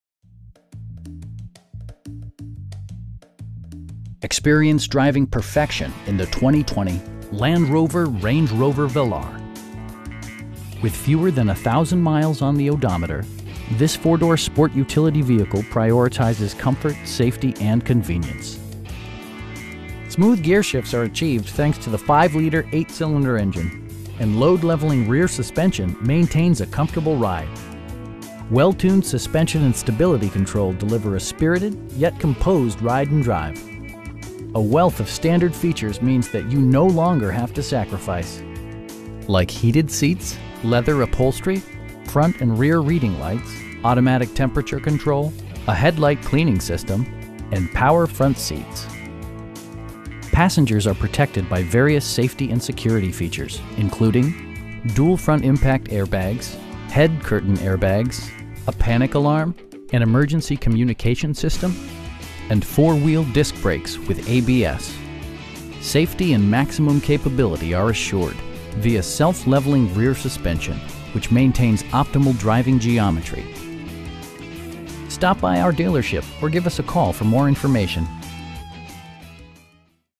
2020 Land Rover Range Rover Velar in Orlando, FL 32810
199 South Lake Destiny Dr

Experience driving perfection in the 2020 Land Rover Range Rover Velar. With fewer than a thousand miles on the odometer, this 4 door sport utility vehicle prioritizes comfort, safety and convenience. Smooth gearshifts are achieved thanks to the 5 liter 8 cylinder engine, and load leveling rear suspension maintains a comfortable ride. Well tuned suspension and stability control deliver a spirited, yet composed, ride and drive A wealth of standard features means that you no longer have to sacrifice. Like heated seats, leather upholstery, front and rear reading lights, automatic temperature control, a headlight cleaning system, tilt steering wheel, power door mirrors and heated door mirrors, and power front seats. For drivers who enjoy the natural environment, a power moon roof allows an infusion of fresh air. Passengers are protected by various safety and security features, including: dual front impact airbags, head curtain airbags, traction control, brake assist, a panic alarm, an emergency communication system, and 4 wheel disc brakes with ABS. Safety and maximum capability are assured via self leveling rear suspension, which maintains optimal driving geometry. You will have a pleasant shopping experience that is fun, informative, and never high pressured. Stop by our dealership or give us a call for more information.